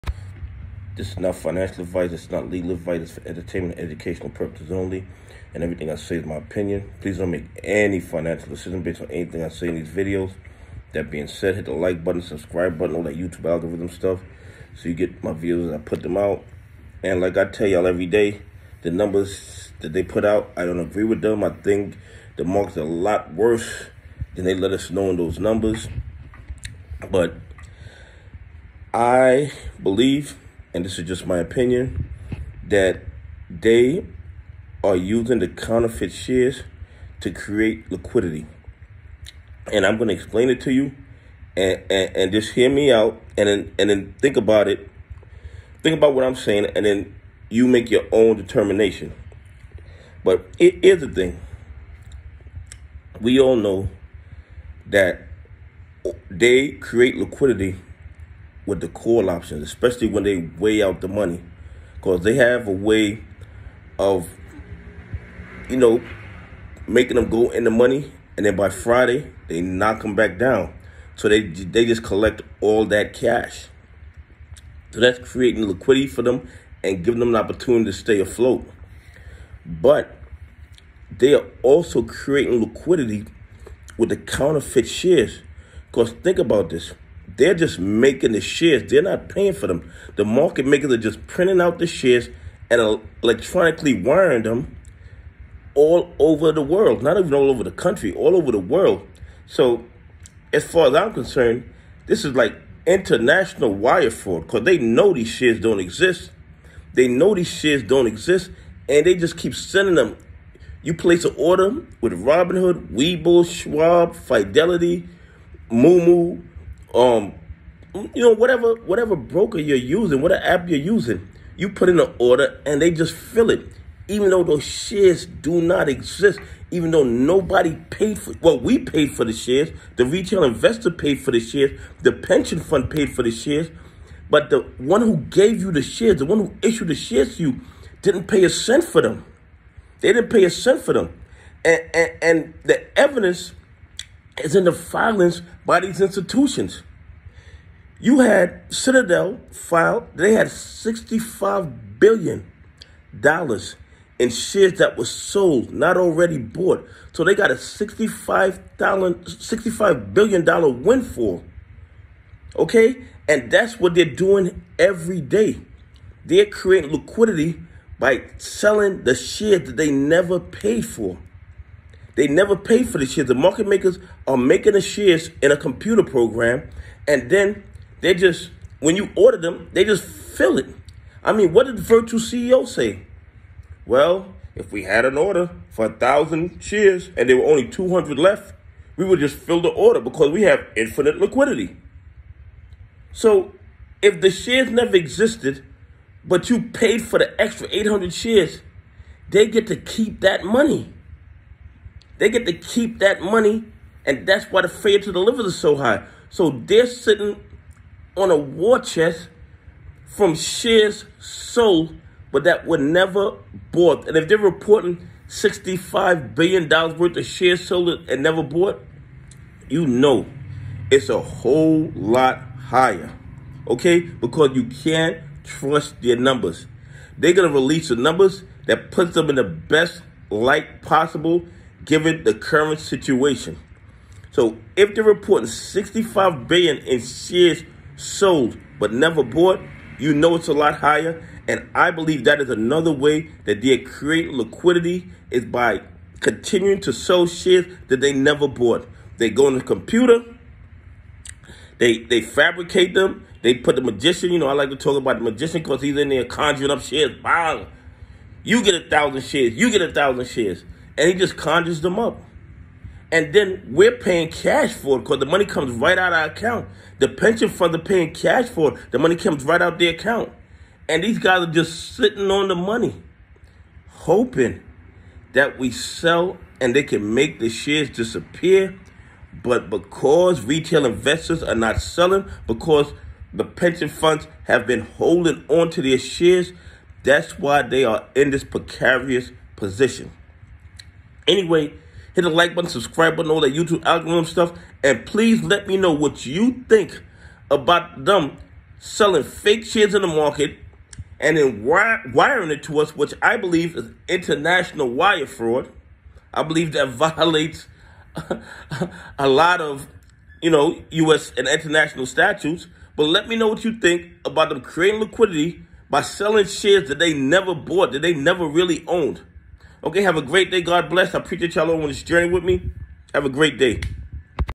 They create liquidity by selling counterfeit shares. Is it wire fraud?
Citadel reports $65,000,000,000 in shares sold but never bought I believe if they reporting these numbers the real numbers are even higher They have $65 Billion in liquidity by selling counterfeit shares